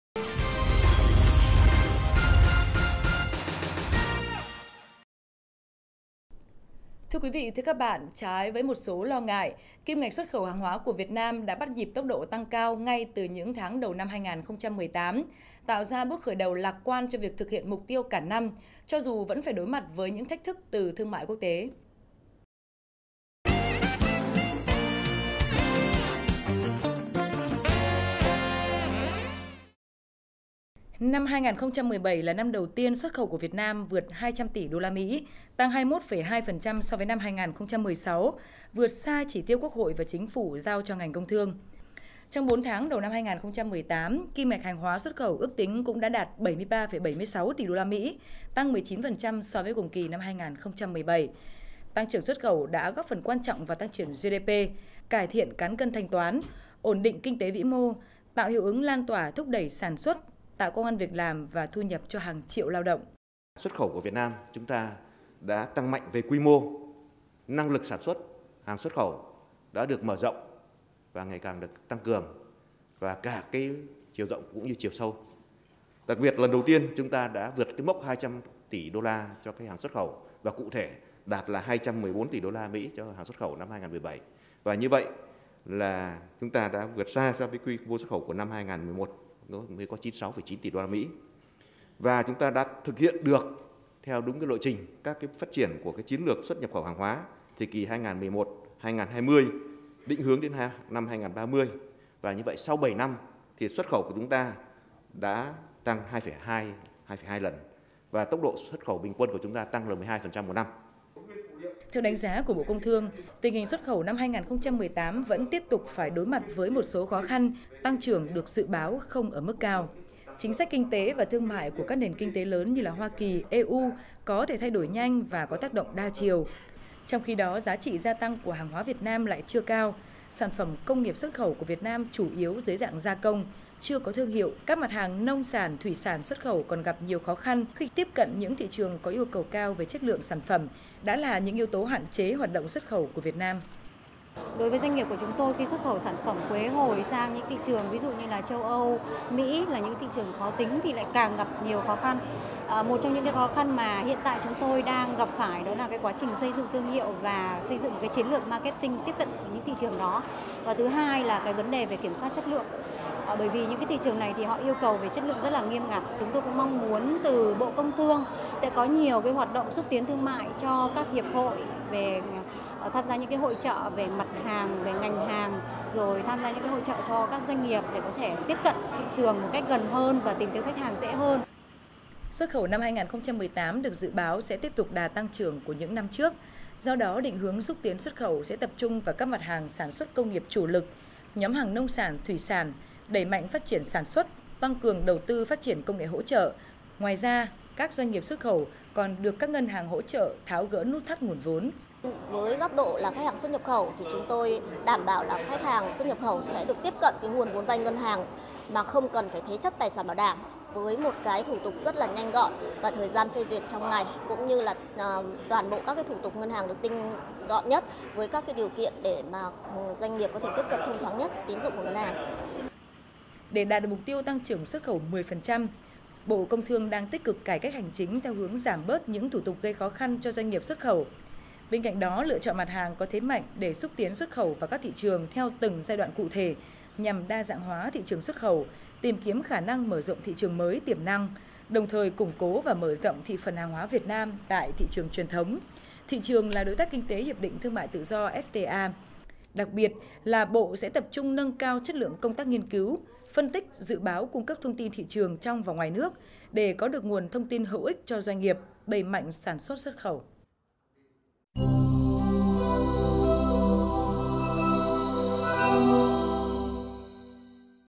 Hướng đi bền vững cho xuất khẩu Việt Nam
Trái với một số lo ngại, kim ngạch xuất khẩu hàng hóa của Việt Nam đã bắt nhịp tốc độ tăng cao ngay từ những tháng đầu của năm 2018, tạo ra khởi đầu lạc quan cho việc thực hiện mục tiêu cả năm cho dù vẫn  phải đối mặt với thách thức từ thương mại quốc tế...